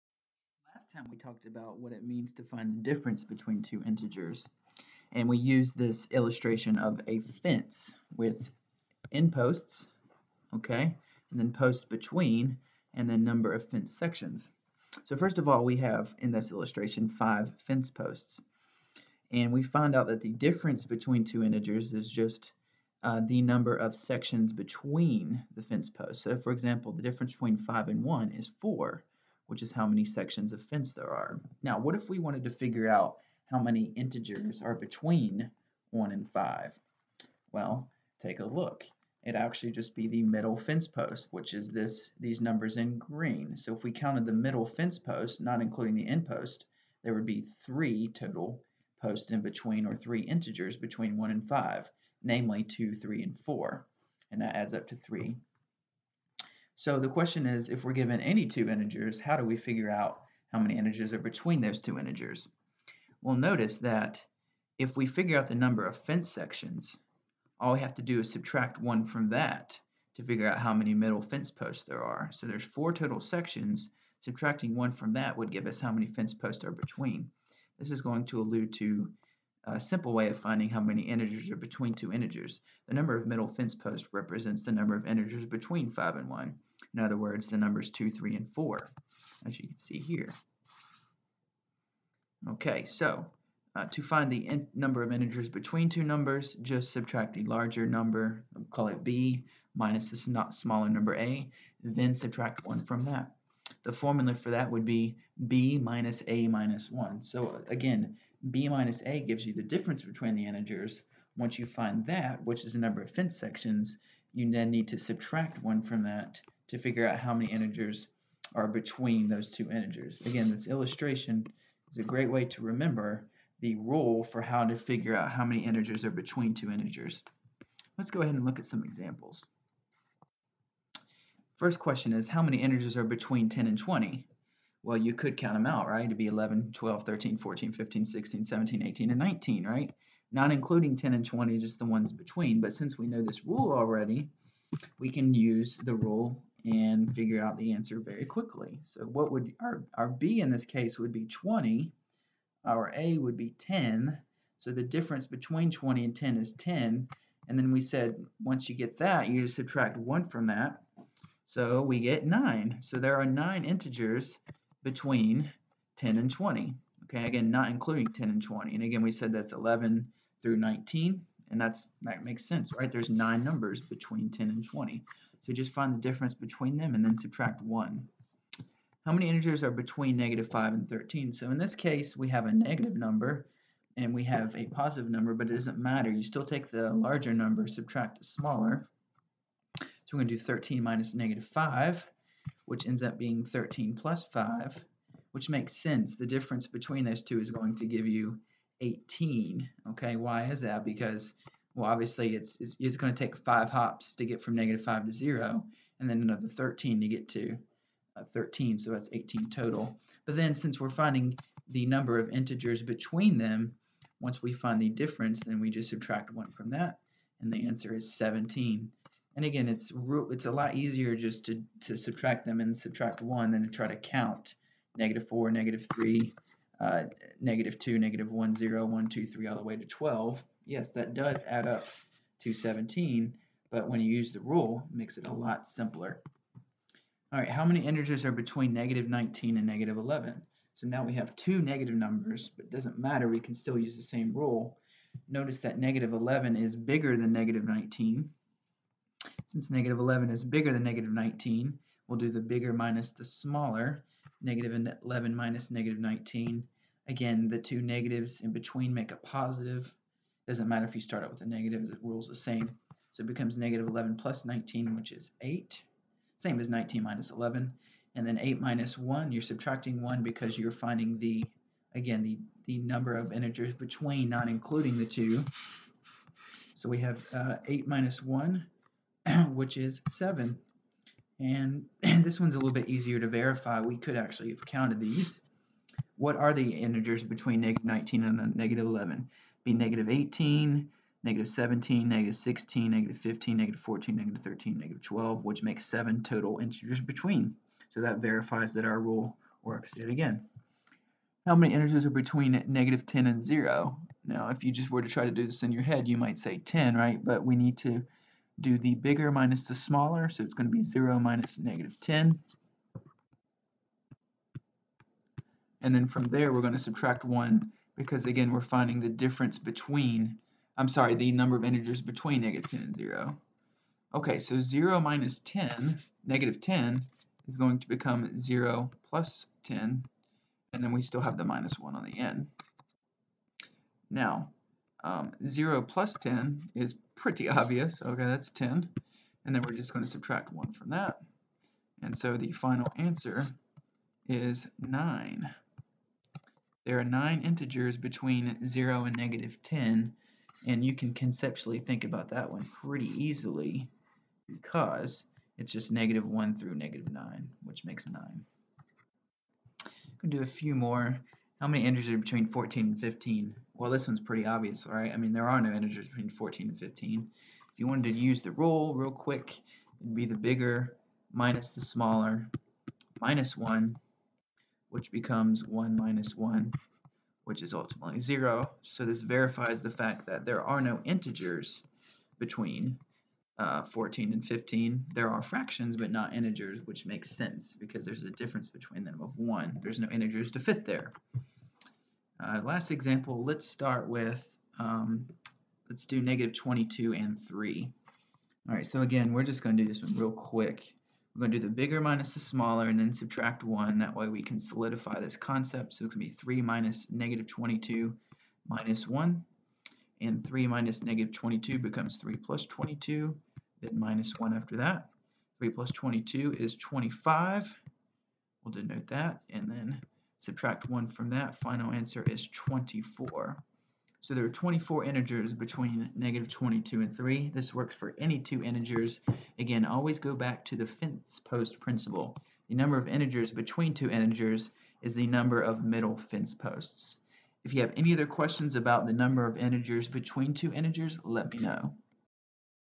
How Many Integers are Between?
How to Figure Out How many numbers are between two numbers with visual illustrations and examples!